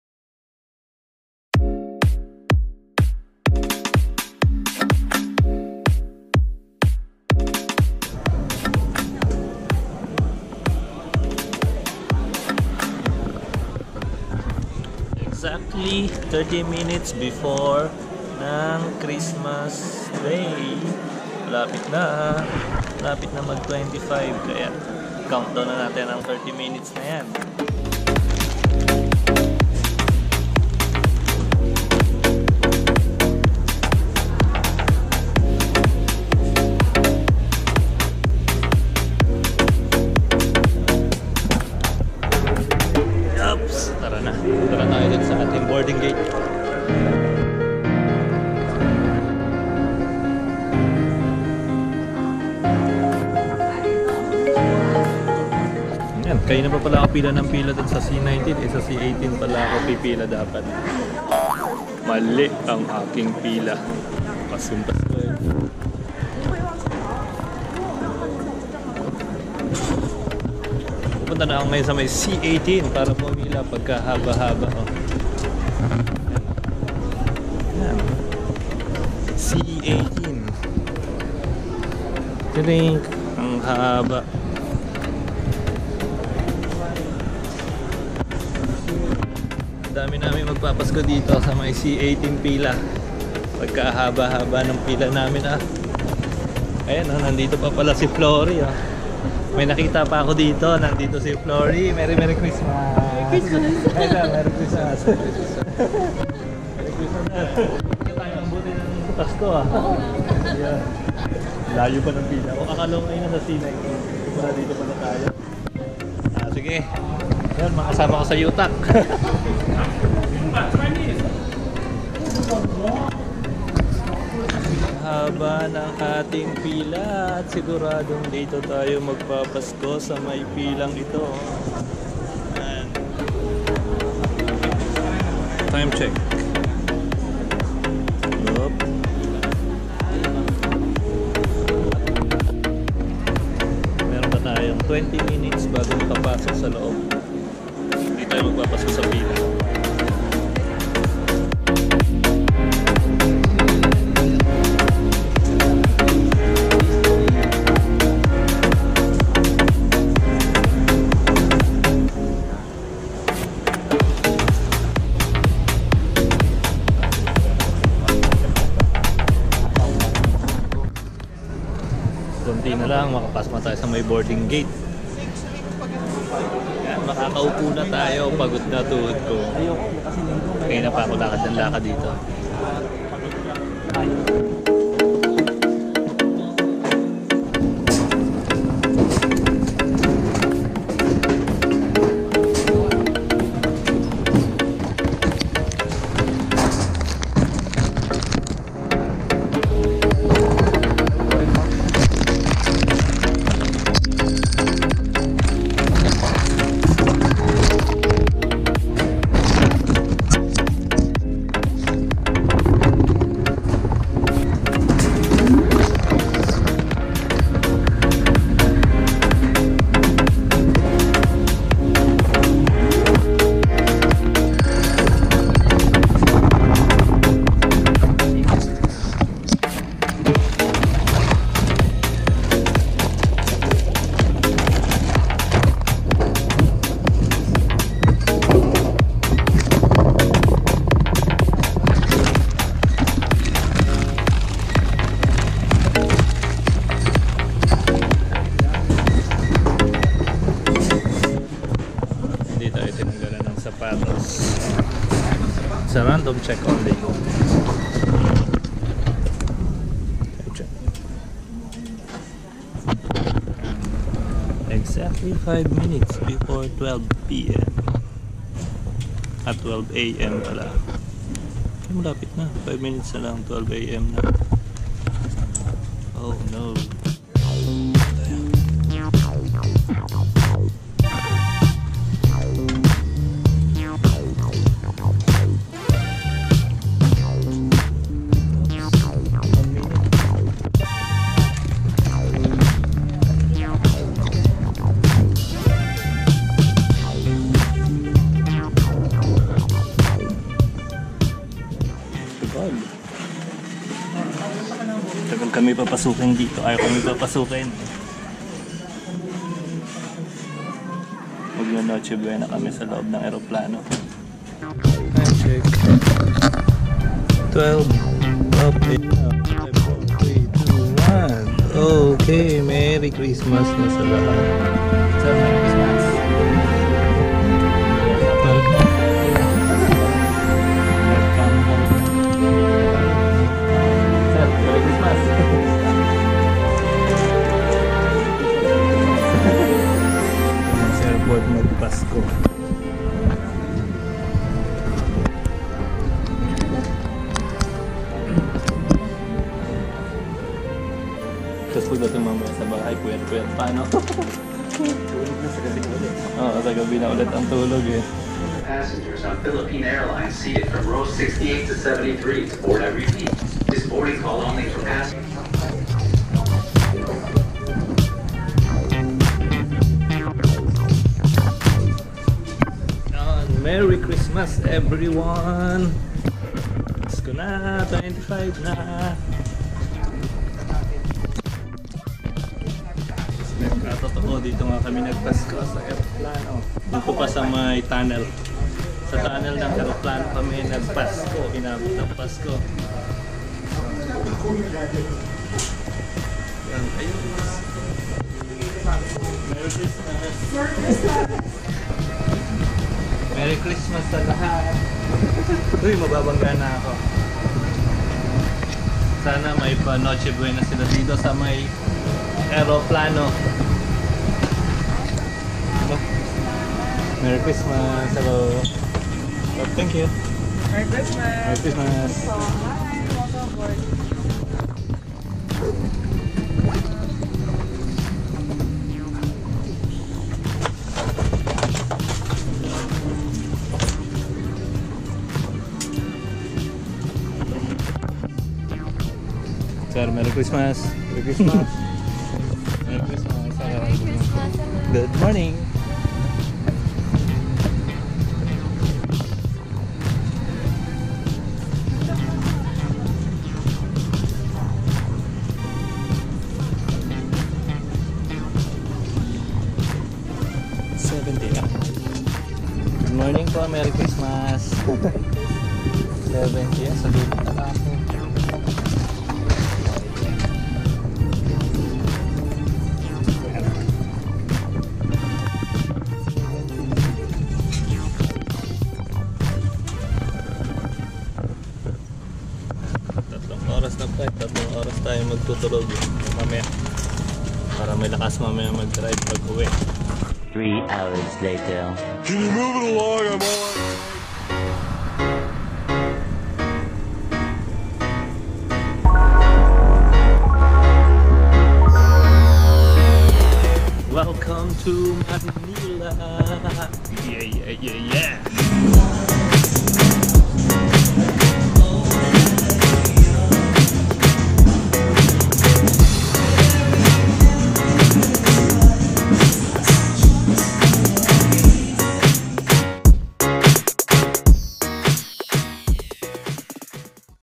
NAGPASKO SA AIRPORT BOARDING GATE | OFW CHRISTMAS SA AIRPORT TERMINAL
Inabutan ng pasko while waiting sa flight. Ito ang mga nangyari.. have a Merry Christmas and a Happy New year to all!!

Share the love❤️ and positivity 😊